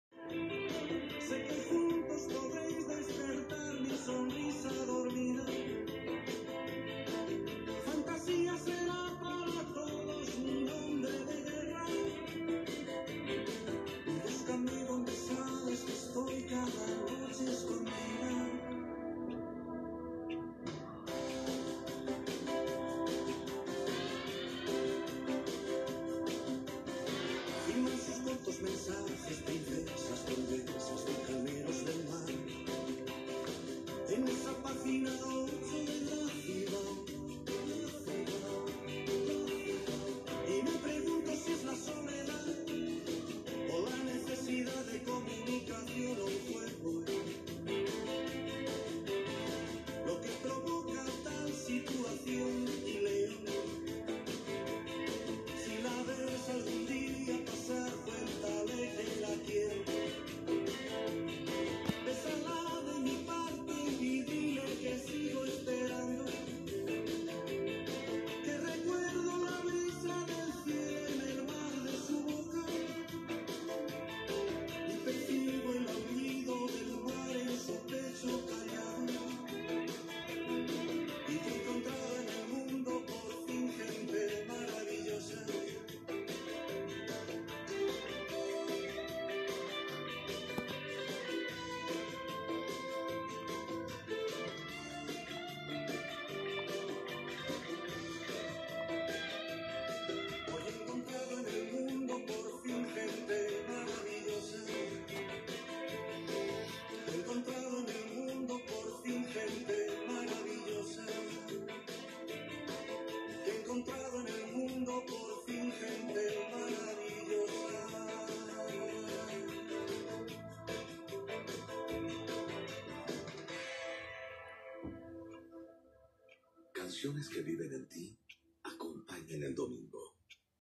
Mis Pies Con Calcetines Grises Con Punteras Negras Reposando y Escuchando Radio Imagina
Es cama reposando el almuerzo y la mejor música en Imagina.

Cuídense amigos, ya en fase 4.